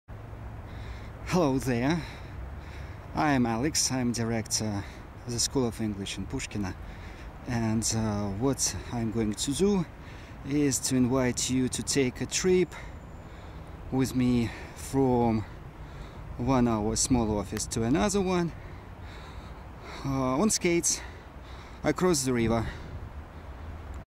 The School of English in Pushkino
From home to the office on skates!
The idea to make a movie originally came a couple of days ago when I really did the trip there and back. I was born on that river and I remember not more than 5 times when the river covered with ice and there was no snow for a week or so. When the biggest in Europe skating rink was open in Moscow and there was not enough place for all people (because more than 5,000 people came) here in Pushkino we broke the record and anyone can skate for free! You are welcome!